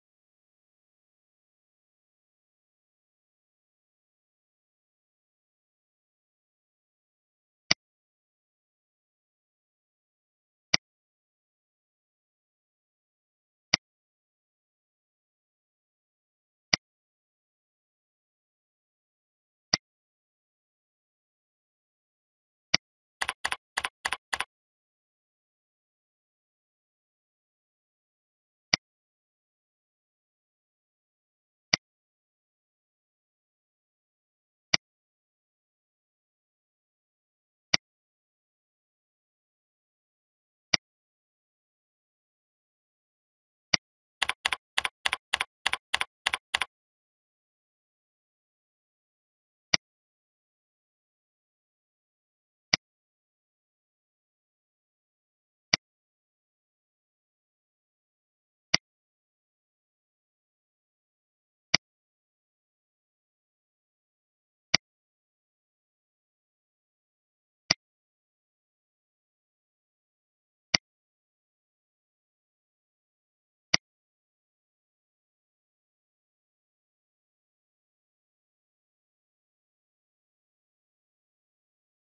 Work With Advance Action
In this demo, we will learn how to work with advance action in Adobe Captivate 5.5.